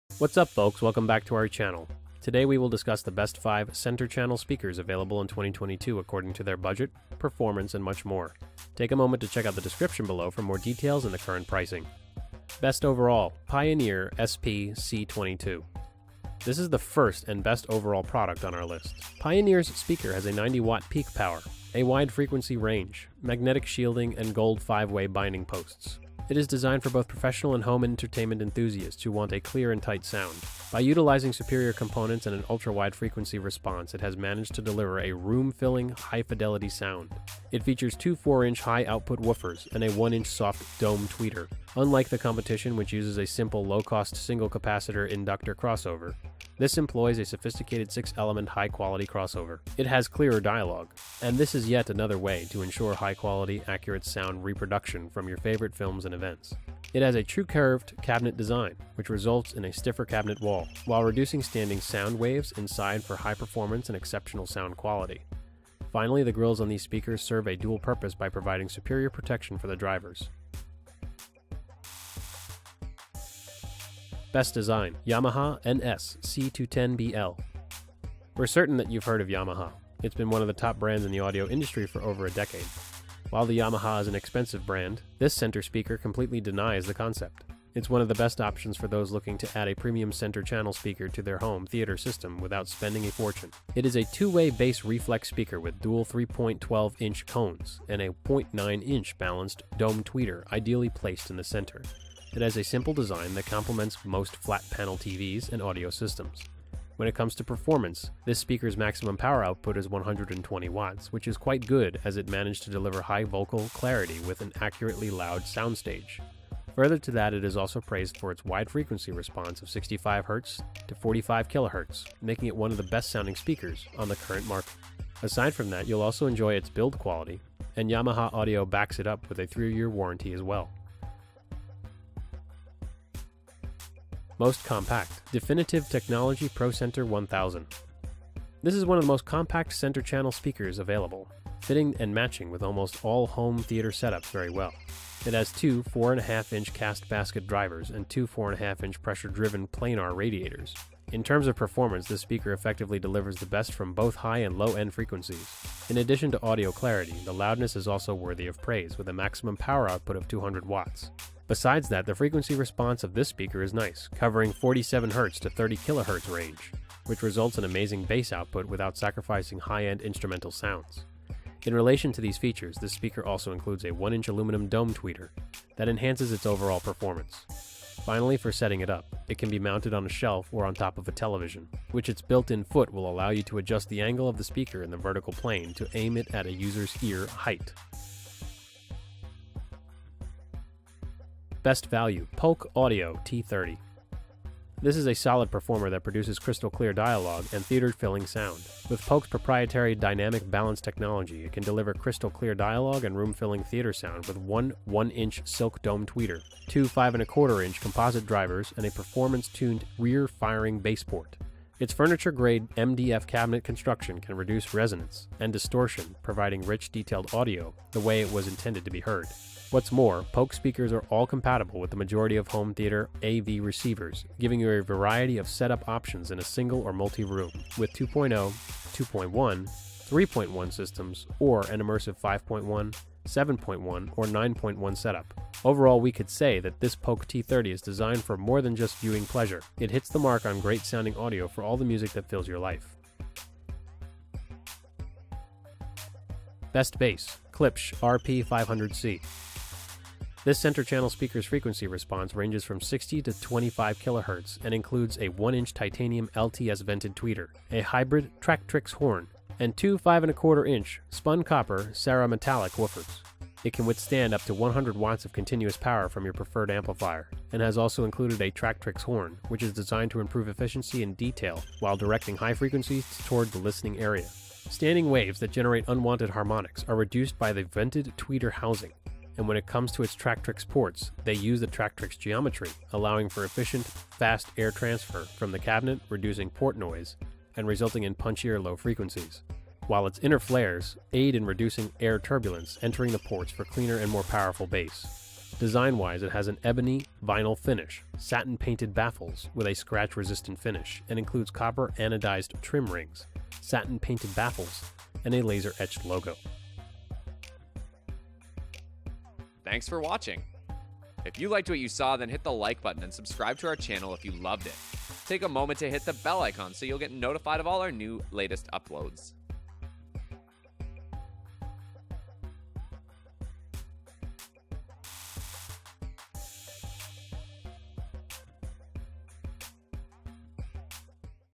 TOP 5: Best Center Channel Speaker 2022 | Create a more cinematic sound experience!
Check Out The Best Center Channel Speaker Here!
► Best Overall-Pioneer SP-C22
► Best Design-Yamaha NS-C210BL
► Most Compact-Definitive Technology Procenter 1000
► Best Value-Polk Audio T30
► Best Bass-Kilpsch RP-500C

VIDEO GUIDE
00:00 Best Center Channel Speaker
00:12 Best Overall
01:14 Best Design
02:14 Most Compact
03:11 Best Value
04:06 Best Bass

Pioneer SP-C22 is the first and the best overall product on our list. Pioneer's SP-C22 Center Channel Speaker has a 90W peak power, a wide frequency range, magnetic shielding, and gold 5-way binding posts. It is designed for both professional and home entertainment enthusiasts who want a clear and tight sound. And, by utilizing superior components and an ultra-wide frequency response, it has managed to deliver a room-filling, high-fidelity sound. It features two 4-inch high-output woofers and a 1-inch soft dome tweeter. And unlike the competition, which uses a simple, low-cost single capacitor/inductor crossover, the SP-C22 employs a sophisticated six element high quality crossover. It has clearer dialogue. And this is yet another way to ensure high-quality, accurate sound reproduction from your favorite films and events. It has got a true curved cabinet design, which results in a stiffer cabinet wall, while reducing standing sound waves inside for high performance and exceptional sound quality. Finally, the grills on these speakers serve a dual purpose by providing superior protection for the drivers.

Next is Yamaha NS-C210BL. We're certain that you've heard of Yamaha; it's been one of the top brands in the audio industry for over a decade. While the Yamaha is an expensive brand, this NS-C210BL Center Channel Speaker completely denies that concept. It's one of the best options for those looking to add a premium center channel speaker to their home theater system without spending a fortune. It is a 2-way bass-reflex speaker with dual 3.12 inch cones and a 0.9 inch balanced dome tweeter ideally placed in the center. It has a simple design that complements most flat-panel TVs and audio systems. And when it comes to performance, this speaker's maximum power output is 120 Watts, which is quite good as it managed to deliver a high vocal clarity with an accurately loud sound stage. Further to that, it is also praised for its wide frequency response of 65 Hz - 45 kHz, making it one of the best-sounding speakers on the current market. Aside from that, you'll also enjoy its build quality, and Yamaha Audio backs it up with a 3-year warranty as well.

Next up, we have the Definitive Technology Procenter 1000. This Procenter 1000 is one of the most compact center channel speakers available, fitting and matching with almost all home theater setups very well. It has two 4.5" cast basket drivers and two 4.5" pressure-driven planar radiators. And I n terms of performance, this speaker effectively delivers the best from both high and low-end frequencies. In addition to audio clarity, the loudness is also worthy of praise, with a maximum power output of 200 Watts. Besides that, the frequency response of this speaker is nice, covering the 47Hz-30kHz range, which results in amazing bass output without sacrificing high-end instrumental sounds. In relation to these features, this speaker also includes a 1-inch aluminum dome tweeter, that enhances its overall performance. Finally, for setting it up, I t can be mounted on a shelf or on top of a television, while its built-in foot will allow you to adjust the angle of the speaker in the vertical plane to aim it at the user's ear height.

Next is Polk Audio T30. This T30 is a solid performer that produces crystal-clear dialogue and theater-filling sound. With  Polk's proprietary Dynamic Balance technology,